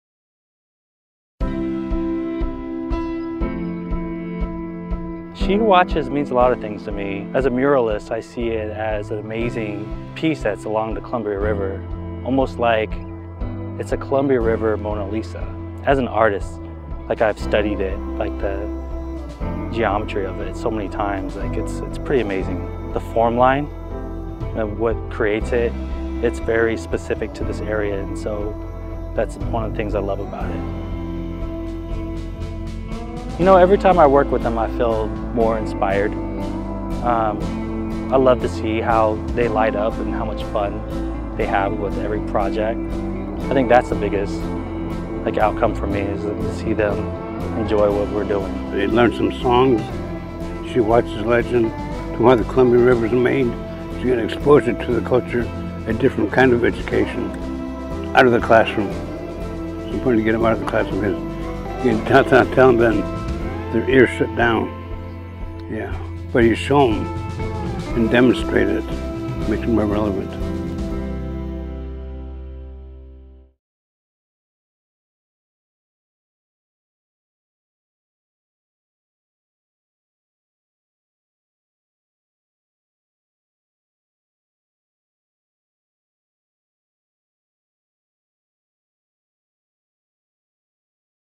Students visit She Who Watches through Confluence Outdoors
Artist Toma Villa (Yakama) and storyteller Ed Edmo (Shoshone-Bannock, Yakama, Nez Perce) recently worked with students from White Salmon, Washington through Confluence Outdoors. The students visited Columbia Hills State Park, home of the famous petroglyph, She Who Watches. They learned about the Indigenous cultures of the Columbia River and took part in an extraordinary art project. Watch and enjoy and please share this with friends, family, colleagues and especially teachers!